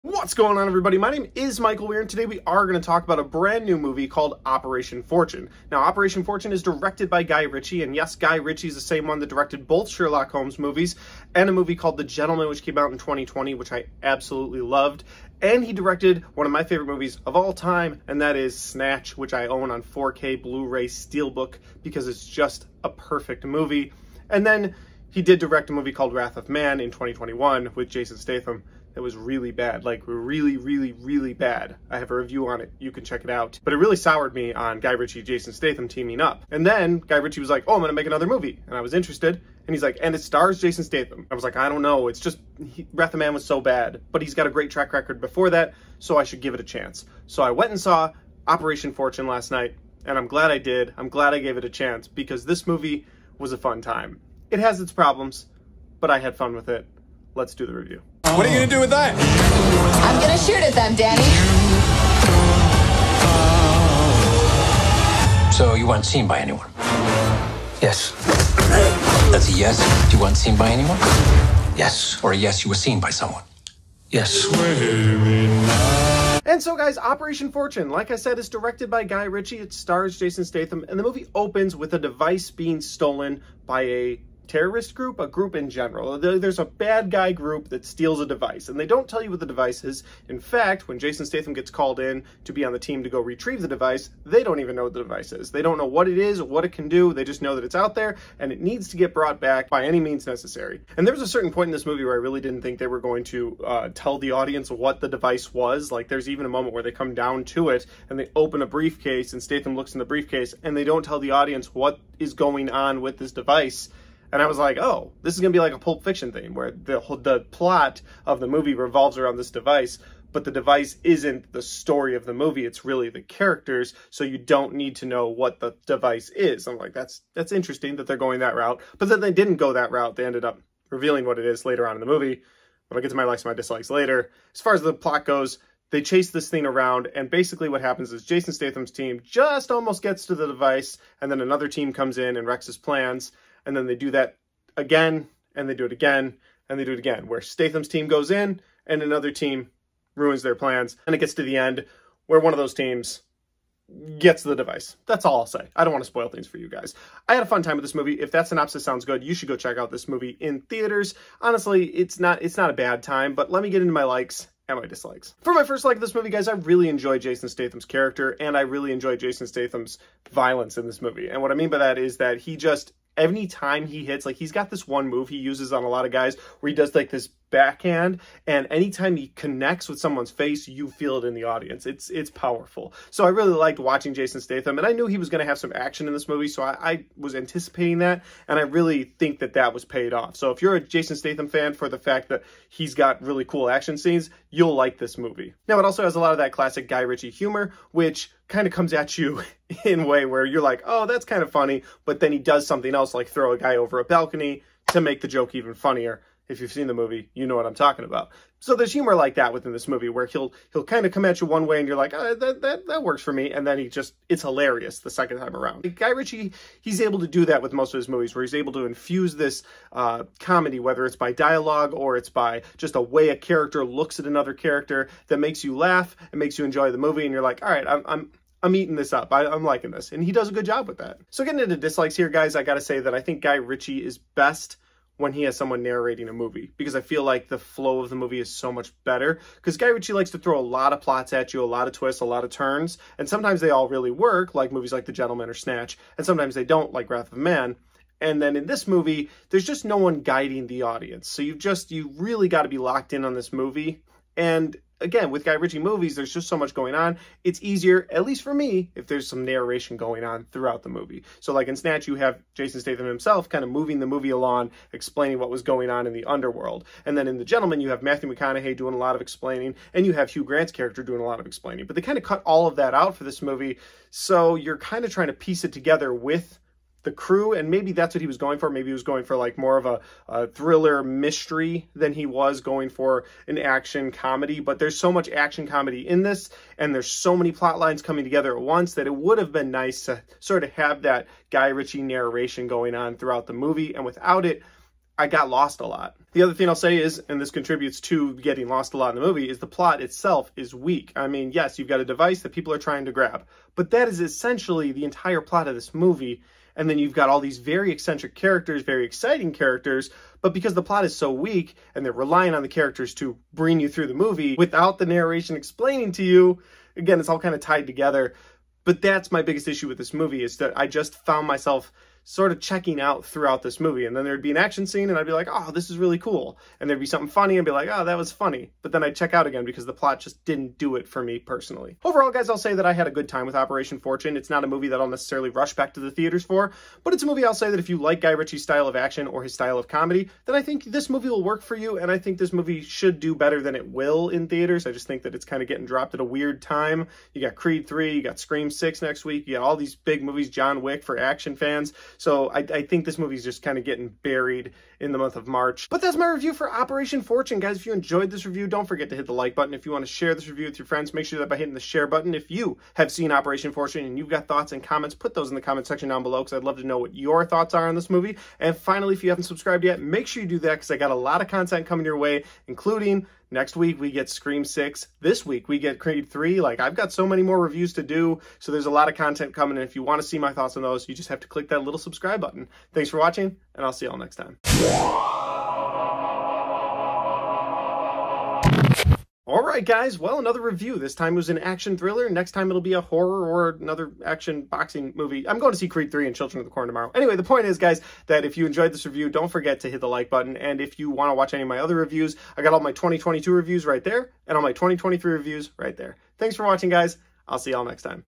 OPERATION FORTUNE: MOVIE REVIEW
Guy Ritchie gave us a confusing yet enjoyable mess of a movie.

There were some fun performances but not enough plot to keep me invested the entire time.